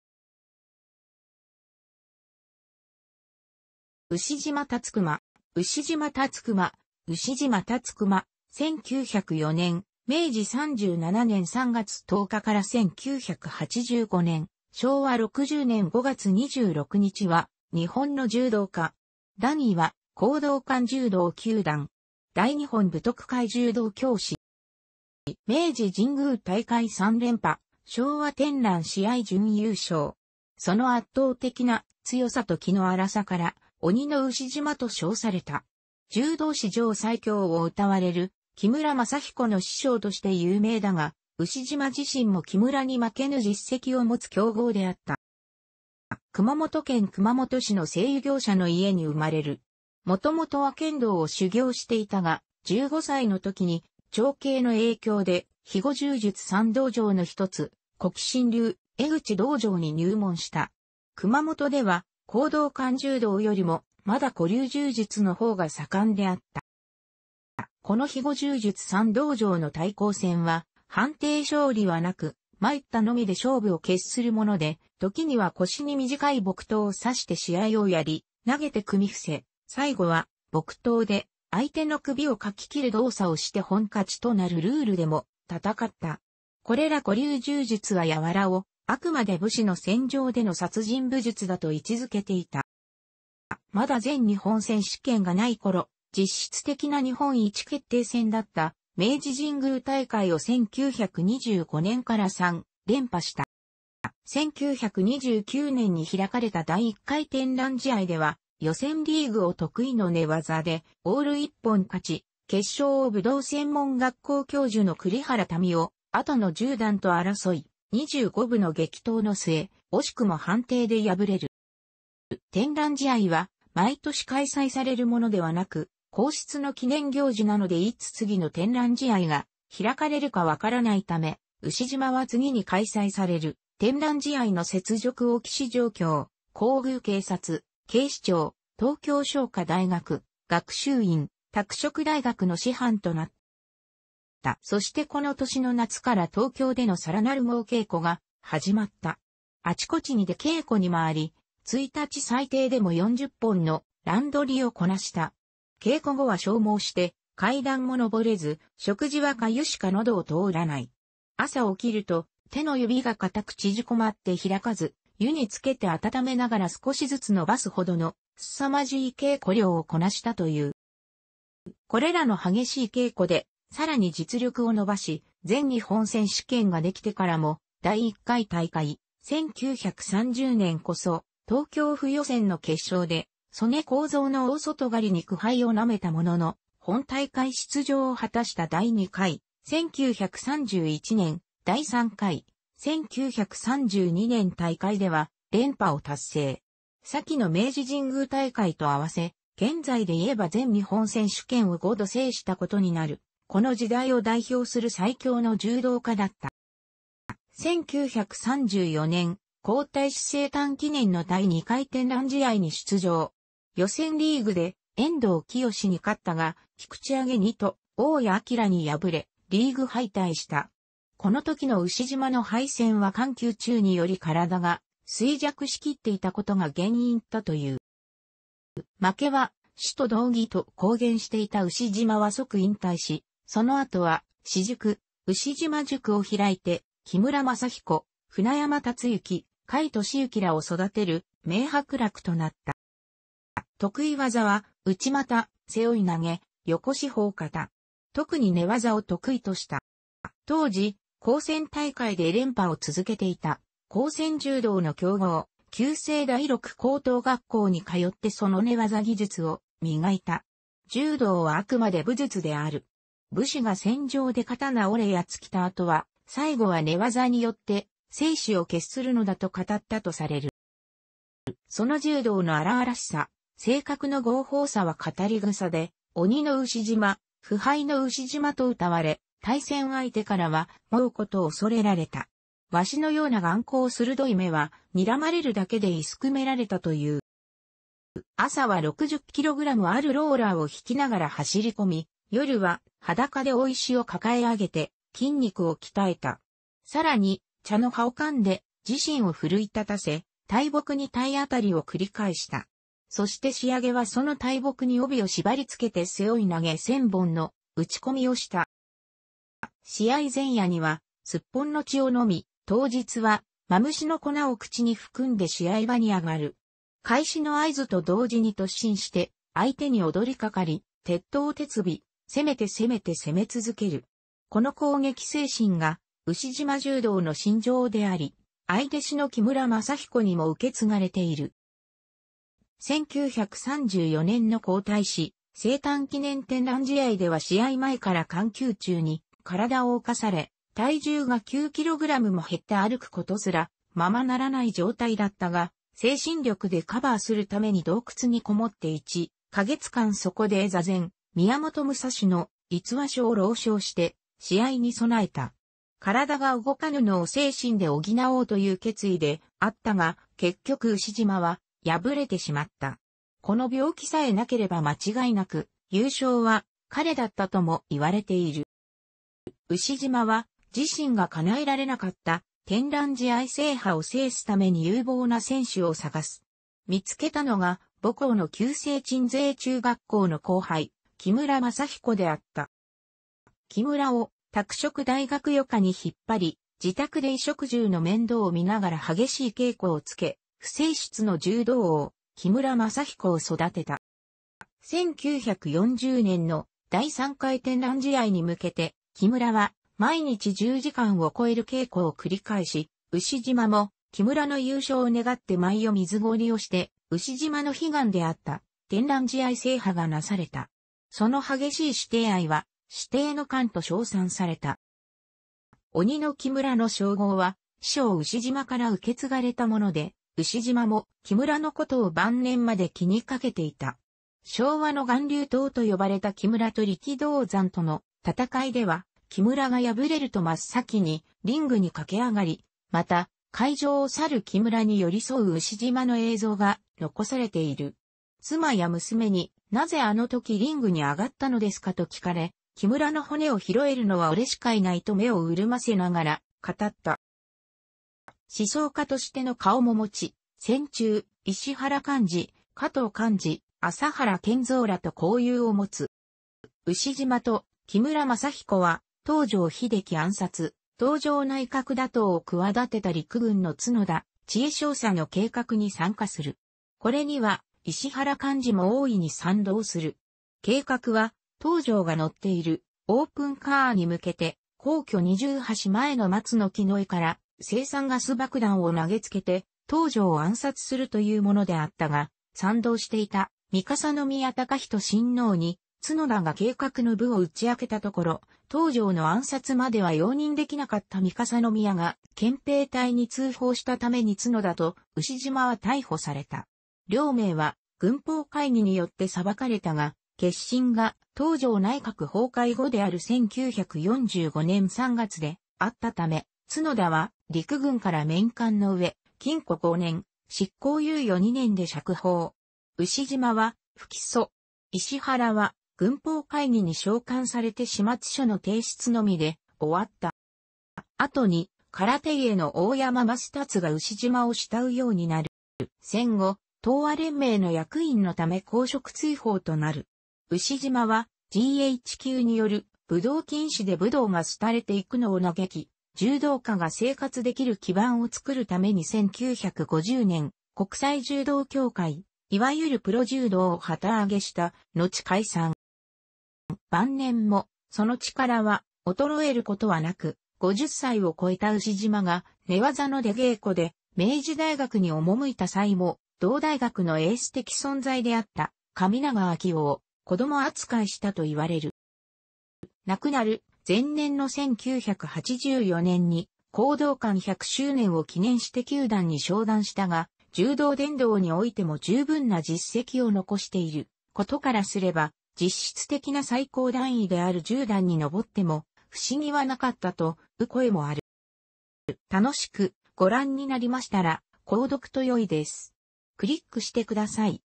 牛島辰熊
牛島 辰熊（うしじま たつくま、1904年（明治37年）3月10日 - 1985年（昭和60年）5月26日）は、日本の柔道家。段位は講道館柔道九段。大日本武徳会柔道教士。

明治神宮大会3連覇、昭和天覧試合準優勝。その圧倒的な強さと気の荒さから「鬼の牛島」と称された。柔道史上最強を謳われる木村政彦の師匠として有名だが、牛島自身も木村に負けぬ実績を持つ強豪であった。

熊本県熊本市の製油業者の家に生まれる。元々は剣道を修行していたが、15歳の時に長兄の影響で肥後柔術三道場の一つ、扱心流江口道場に入門した。熊本では講道館柔道よりも、まだ古流柔術の方が盛んであった。

この肥後柔術三道場の対抗戦は、判定勝利はなく「参った」のみで勝負を決するもので、時には腰に短い木刀を差して試合をやり、投げて組み伏せ、最後は木刀で相手の首を掻き斬る動作をして一本勝ちとなるルールでも戦った。これら古流柔術は柔（やわら）をあくまで武士の戦場での殺人武術だと位置づけていた。

まだ全日本選士権がない頃、実質的な日本一決定戦だった明治神宮大会を1925年から3連覇した。

1929年に開かれた第1回天覧試合では予選リーグを得意の寝技でオール一本勝ち、決勝を武道専門学校教授の栗原民雄（後の十段）と争い、25分の激闘の末、惜しくも判定で敗れる。

天覧試合は毎年開催されるものではなく、皇室の記念行事なのでいつ次の天覧試合が開かれるか分からないため、牛島は次に開催される天覧試合の雪辱を期し上京、皇宮警察、警視庁、東京商科大学、学習院、拓殖大学の師範となった。

そしてこの年の夏から東京での更なる猛稽古が始まった。あちこちに出稽古に回り、1日最低でも40本の乱取りをこなした。稽古後は消耗して階段も昇れず、食事は粥しか喉を通らない。朝起きると手の指が固く縮こまって開かず、湯につけて暖めながら少しずつ伸ばすほどの凄まじい稽古量をこなしたという。

これらの激しい稽古でさらに実力を伸ばし、全日本選士権ができてからも第1回大会（1930年）こそ東京府予選の決勝で曽根幸蔵の大外刈に苦杯を嘗めたものの、本大会出場を果たした第2回（1931年）・第3回（1932年）大会では連覇を達成。先の明治神宮大会と合わせ、現在で言えば全日本選手権を5度制した事になる、この時代を代表する最強の柔道家だった。

1934年、皇太子生誕記念の第2回天覧試合に出場。予選リーグで遠藤清に勝ったが菊池揚二と大谷晃に敗れ、リーグ敗退した。この時の牛島の敗戦は肝吸虫により体が衰弱しきっていた事が原因だったという。

「負けは死と同義」と公言していた牛島は即引退し、その後は私塾「牛島塾」を開いて木村政彦、船山辰幸、甲斐利之らを育てる名伯楽となった。

得意技は内股、背負い投げ、横四方固。特に寝技を得意とした。

当時、高専大会で連覇を続けていた高専柔道の強豪・旧制第六高等学校に通ってその寝技技術を磨いた。「柔道はあくまで武術である。武士が戦場で刀折れ矢尽きたあとは、最後は寝技によって生死を決するのだ」と語ったとされる。

その柔道の荒々しさ、性格の豪放さは語り草で、「鬼の牛島」「不敗の牛島」と謳われ、対戦相手からは「猛虎」と恐れられた。鷲のような眼光鋭い目は、睨まれるだけで射すくめられたという。

朝は60kgあるローラーを牽きながら走り込み、夜は裸で大石を抱え上げて筋肉を鍛えた。さらに茶の葉を噛んで自身を奮い立たせ、大木に体当たりを繰り返した。そして仕上げはその大木に帯を縛り付けて背負い投げ千本の打ち込みをした。

試合前夜にはスッポンの血を飲み、当日はマムシの粉を口に含んで試合場に上がる。開始の合図と同時に突進して相手に躍りかかり、徹頭徹尾、攻めて攻めて攻め続ける。この攻撃精神が牛島柔道の信条であり、愛弟子の木村政彦にも受け継がれている。

1934年の皇太子生誕記念天覧試合では試合前から肝吸虫に体を冒され、体重が9kgも減って歩く事すらままならない状態だったが、精神力でカバーするために洞窟に籠もって1カ月間そこで坐禅し、宮本武蔵の『五輪書』を朗唱して試合に備えた。体が動かぬのを精神で補おうという決意であったが、結局牛島は敗れてしまった。この病気さえなければ間違いなく優勝は彼だったとも言われている。

牛島は自身が叶えられなかった天覧試合制覇を制すために有望な選手を探す。見つけたのが...